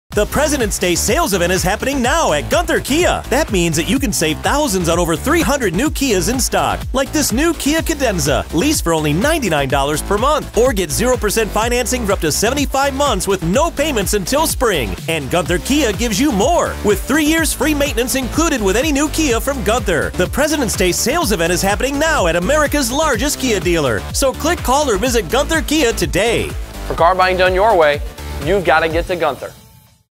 Presidents Day Sale 2018 at Gunther Kia Fort Lauderdale
- The Presidents Day Sales Event is happening now at Gunther Kia. That means that you can save thousands on over 300 new Kias in stock. Like this new Kia Cadenza lease for only $99 per month. Or get 0% financing for up to 75 months with no payments until spring. And Gunther Kia gives you more! With 3 years free maintenance included with any new Kia from Gunther. The Presidents Day Sales Event is happening now at America’s Largest Kia Dealer. So Click, Call, or Visit Gunther Kia today. For car buying done your way, you’ve got to get to Gunther.